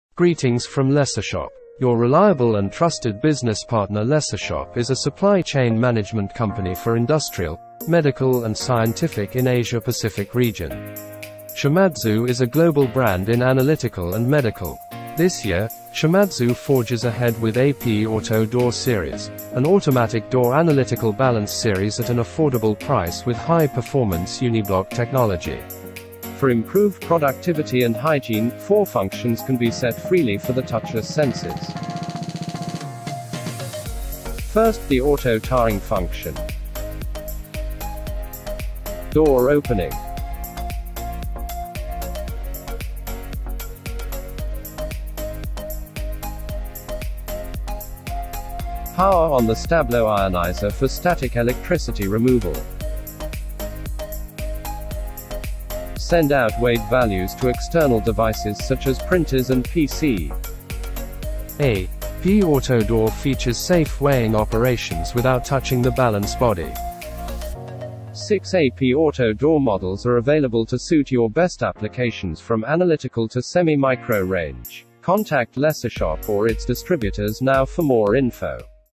Shimadzu AP W-AD
Advanced Performance UniBloc Balances
Provides High-Speed Response and High Stability
The new AP-AD series models not only offer excellent basic performance, such as fast weighing times of about two seconds and high measurement stability that minimizes errors, but also include new functionality that improves convenience, such as automatic doors and touchless sensors.
New automatic door functionality makes weighing operations even more convenient.